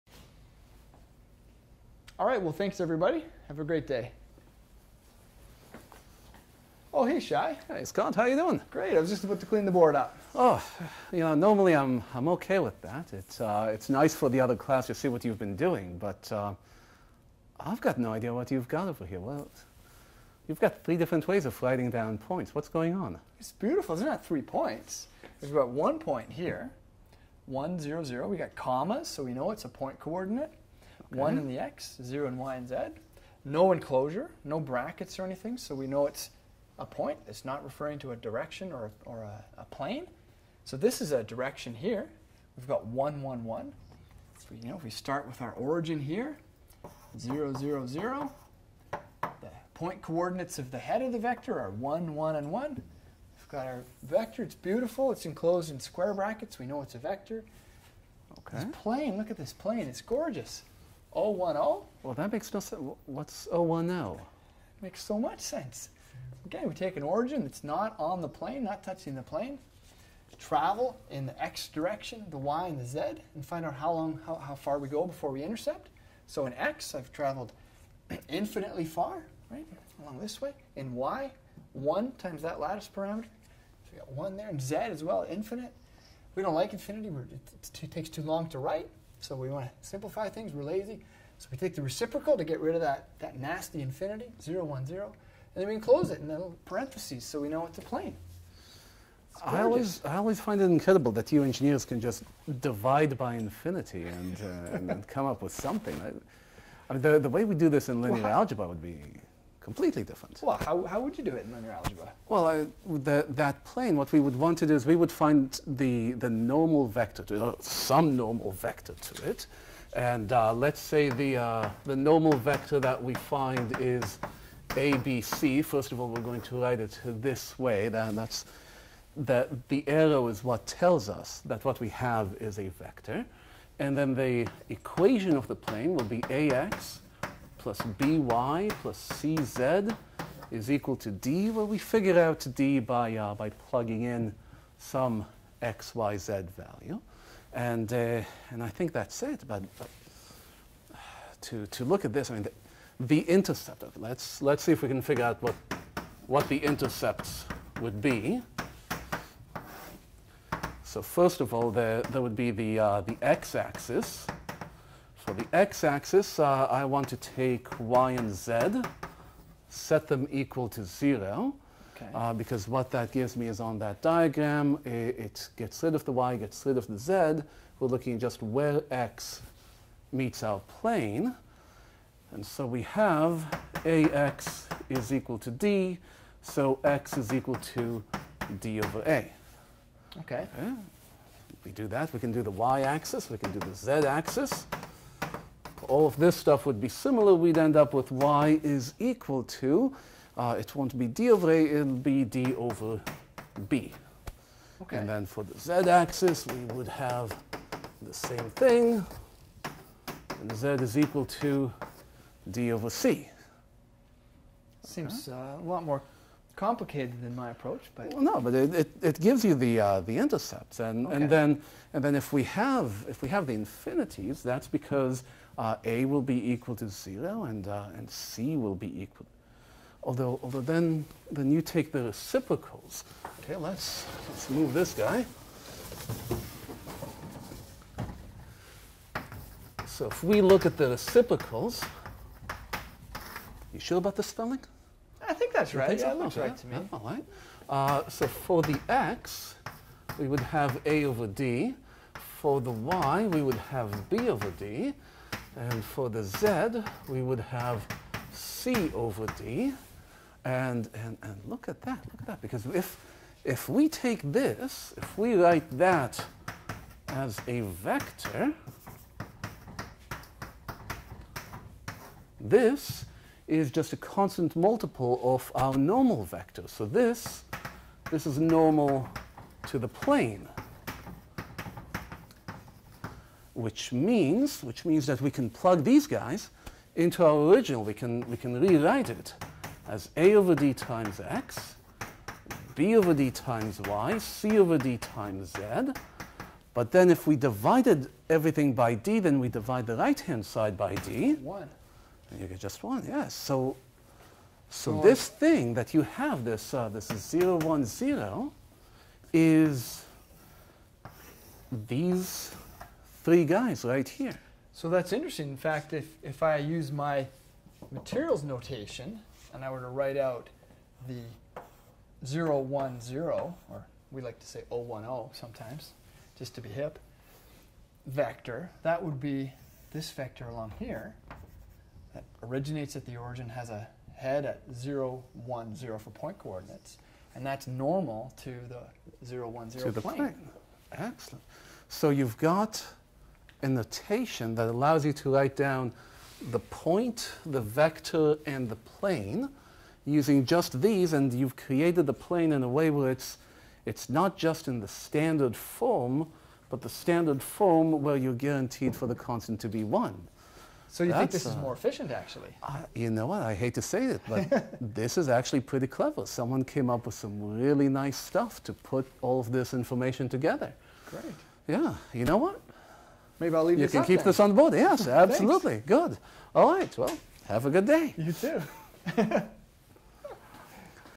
Scott and Shai Duel
In this fun video Scott and Shai Cohen discuss the different notation for planes and directions in their respective fields of materials science and mathematics.

Many thanks to Matthieu Gagne for his work filming and editing this video.